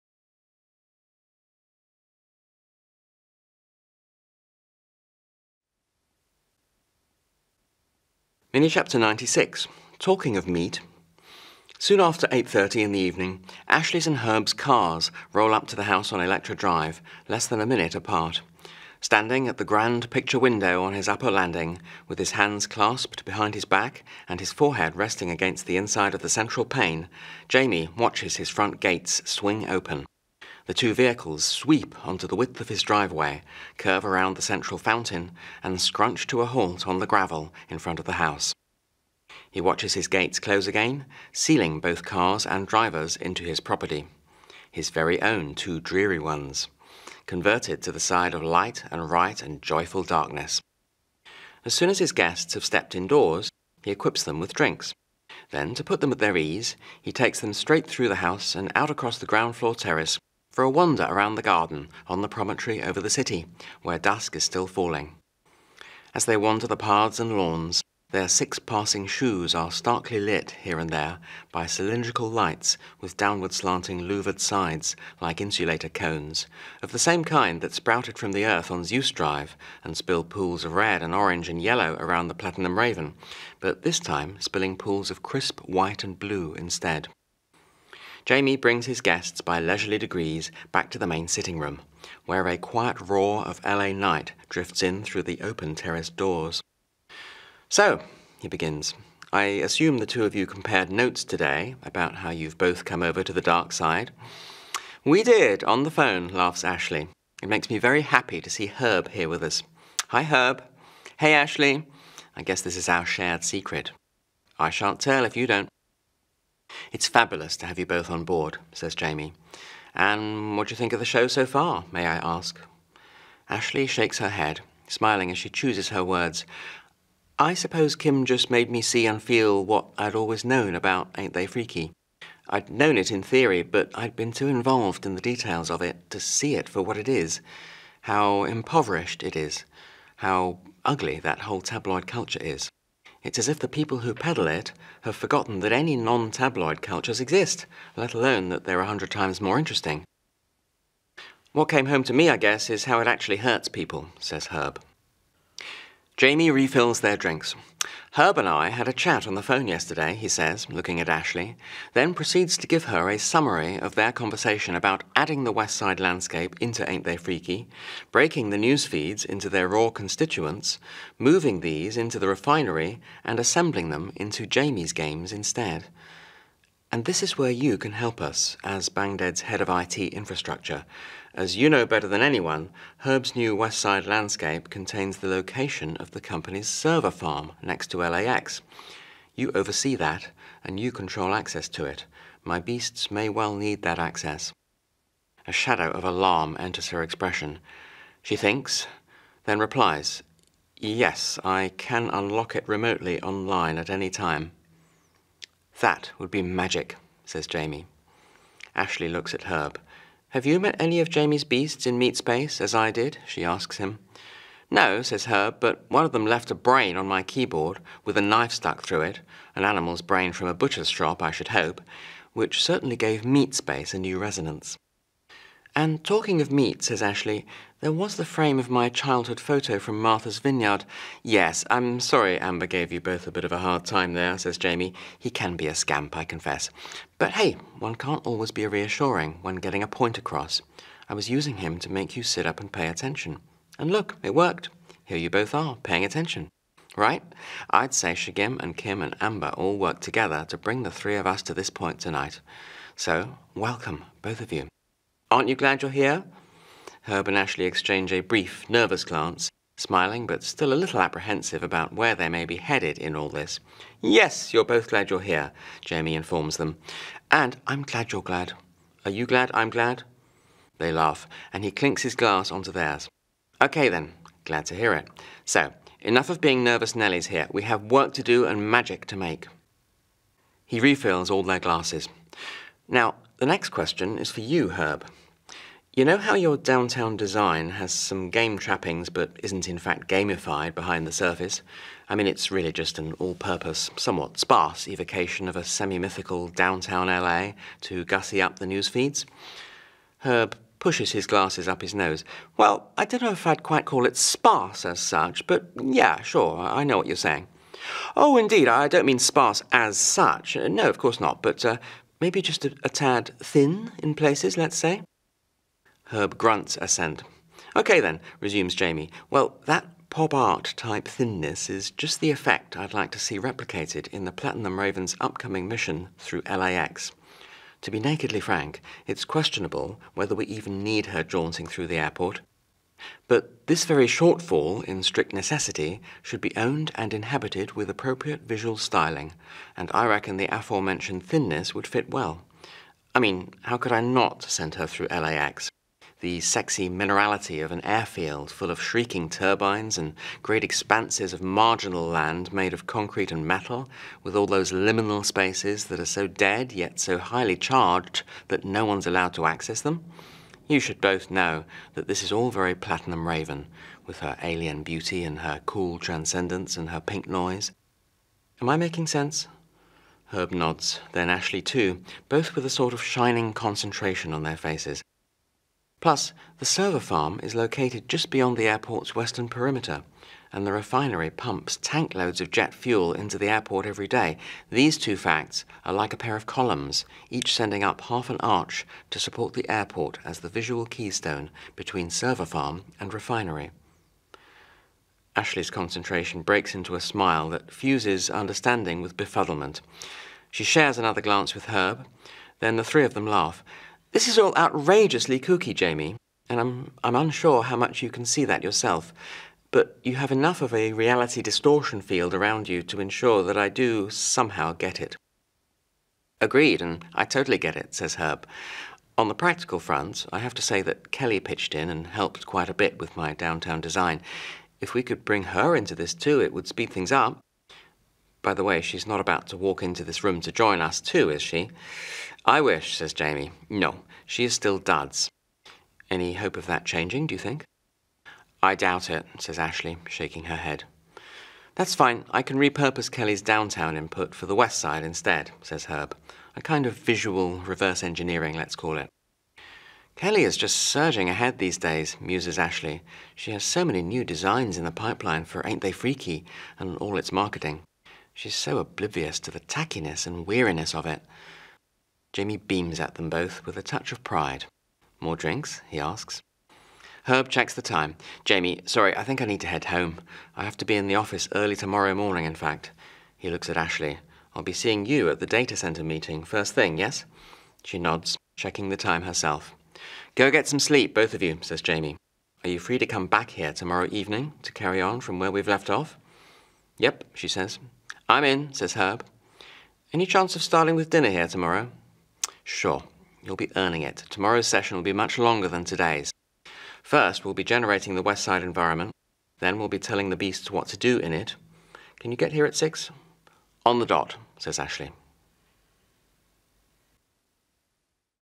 Rohan Quine, “The Beasts of Electra Drive”, 96 Talking of meat
Soon after eight-thirty in the evening, Ashley’s and Herb’s cars roll up to the house on Electra Drive, less than a minute apart. Standing at the grand picture-window on his upper landing, with his hands clasped behind his back and his forehead resting against the inside of the central pane, Jaymi watches his front gates swing open. The two vehicles sweep into the width of his driveway, curve around the central fountain, and scrunch to a halt on the gravel in front of the house.

 He watches his gates close again, sealing both cars and drivers into his property. His very own two Dreary Ones! Converted to the side of light and right and joyful darkness…

 As soon as his guests have stepped indoors, he equips them with drinks. Then to put them at their ease, he takes them straight through the house and out across the ground-floor terrace, for a wander around the garden on the promontory over the city, where dusk is still falling. As they wander the paths and lawns, their six passing shoes are starkly lit, here and there, by cylindrical lights with downward-slanting louvred sides, like insulator cones, of the same kind that sprouted from the earth on Zeus Drive and spilled pools of red and orange and yellow around the Platinum Raven—but this time spilling pools of crisp white and blue instead.

 “Yes, you’re both glad you’re here,” Jaymi informs them. “And I’m glad you’re glad! Are you glad I’m glad?” They laugh, and he clinks his glass onto theirs. “OK then, glad to hear it. So, enough of being nervous nellies here—we have work to do and magic to make!” He refills all their glasses. “Now, the next question is for you, Herb. You know how your Downtown design has some game trappings but isn’t in fact gamified behind the surface? I mean it’s really just an all-purpose, somewhat sparse evocation of a semi-mythical ‘Downtown L.A.’, to gussy up the Newsfeeds?”

 Herb pushes his glasses up his nose. “Well, I don’t know if I’d quite call it sparse, as such. But yeah, sure, I know what you’re saying.”

 “Oh indeed, I don’t mean sparse as such—no, of course not. But maybe just a tad … thin, in places, let’s say?”

 Herb grunts assent.

 “OK then,” resumes Jaymi. “Well, that pop-art-type thinness is just the effect I’d like to see replicated in the Platinum Raven’s upcoming mission through LAX. To be nakedly frank, it’s questionable whether we even need her jaunting through the airport. But this very shortfall in strict necessity should be owned and inhabited with appropriate visual styling, and I reckon the aforementioned thinness would fit well. I mean, how could I not send her through LAX? The sexy minerality of an airfield full of shrieking turbines and great expanses of marginal land made of concrete and metal, with all those liminal spaces that are so dead, yet so highly charged that no one’s allowed to access them … you should both know that this is all very Platinum Raven, with her alien beauty and her cool transcendence and her pink noise. Am I making sense?” Herb nods, then Ashley too, both with a sort of shining concentration on their faces. “Plus, the server farm is located just beyond the airport’s western perimeter, and the refinery pumps tank-loads of jet-fuel into the airport every day: these two facts are like a pair of columns, each sending up half an arch to support the airport as the visual keystone between server farm and refinery.”

 Ashley’s concentration breaks into a smile that fuses understanding with befuddlement. She shares another glance with Herb, then the three of them laugh. “This is all outrageously kooky, Jaymi, and I’m unsure how much you can see that yourself, but you have enough of a reality distortion field around you to ensure that I do, somehow, get it.”

 “Agreed, and I totally get it!” says Herb.

 “Go get some sleep, both of you,” says Jaymi. “Are you free to come back here tomorrow evening, to carry on from where we’ve left off?”

 “Yep,” she says.

 “I’m in,” says Herb. “Any chance of starting with dinner here tomorrow?”

 “Sure. You’ll be earning it: tomorrow’s session will be much longer than today’s. First we’ll be generating the West Side environment, then we’ll be telling the Beasts what to do in it. Can you get here at six?”

 “On the dot,” says Ashley.

And to pick it up from whichever retailer you prefer, the retailers’ links for the audiobook format are at